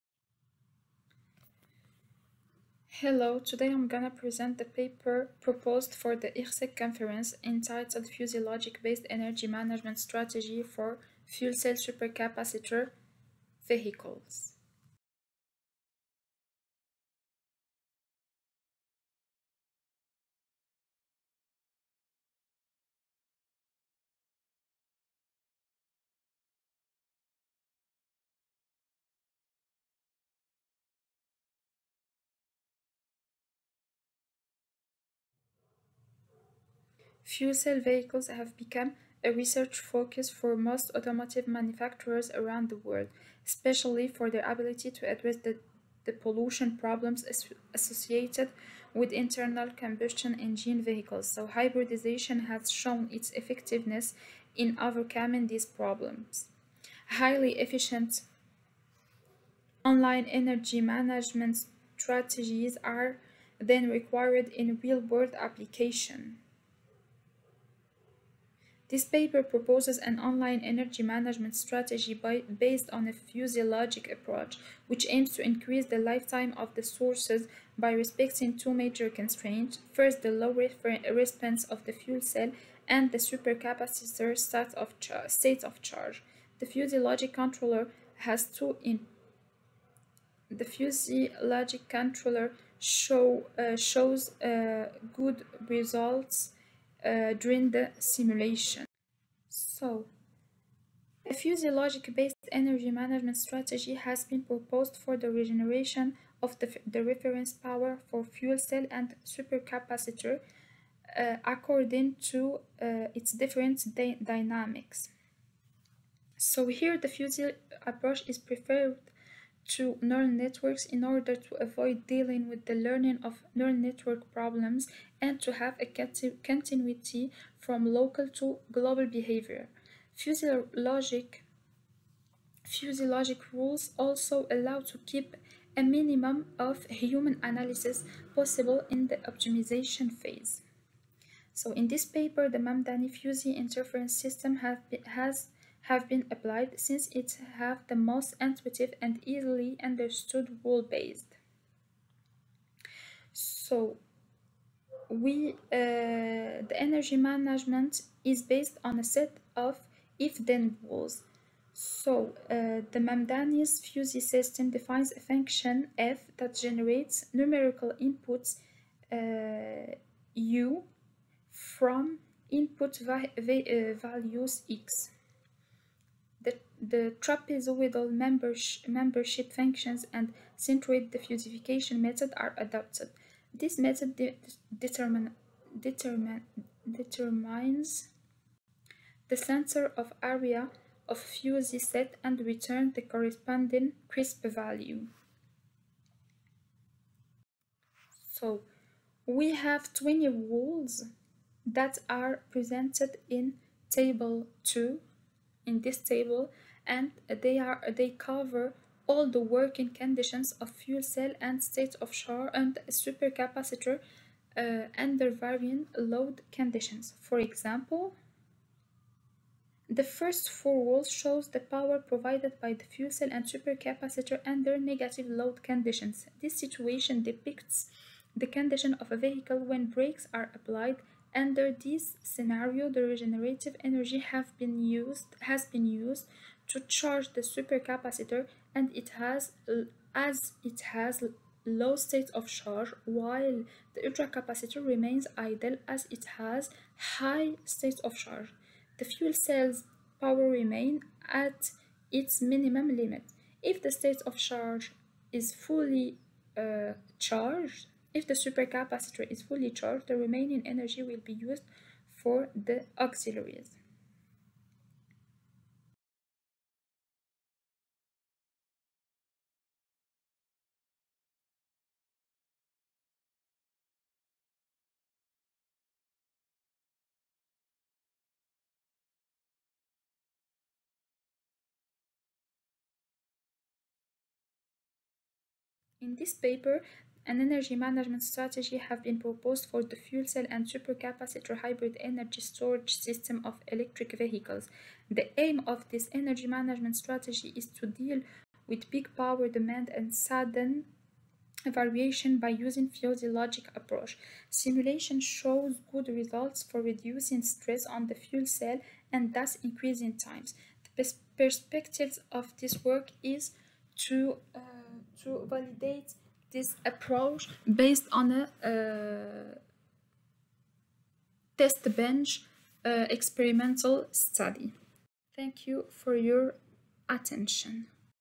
139 - Fuzzy Logic based Energy Management Strategy for Fuel Cell Supercapacitor Hybrid Vehicles
Nada Rifai, Sabor Jalal, Chakib Alaoui, Hamid Gualous
Code: (S6103_ID139)
Paper Title : Fuzzy Logic based Energy Management Strategy for Fuel Cell Supercapacitor Hybrid Vehicles

Presented in:
9th International Renewable and Sustainable Energy Conference
IEEE Conference
Online, November 23-27, 2021
Workshop 6 - Perspectives of Green Hydrogen Development

Presented by :
Nada Rifai
LSMI, ENSAM, University Moulay Ismail, Meknes, Morocco
Meknes, Morocco

Abstract - Fuel cell (FC) vehicles have become a research focus for most automotive manufacturers around the world, especially for their ability to address the pollution problems associated with internal combustion engine vehicles. Hybridization has shown its effectiveness in overcoming these problems. Highly efficient online energy management strategies are then required in real-world applications. This paper proposes an online energy management strategy (EMS), based on a fuzzy logic approach, which aims to increase the lifetime of the sources by respecting two major constraints: the low response of the FC and the Supercapacitor State of Charge (SoC). The fuzzy logic controller has two inputs: the power required for the vehicle traction power and SoC and one output which is the power that the FC must provide. The proposed strategy offers some advantages, such as less pressure on fuel cell therefore higher lifetime for it."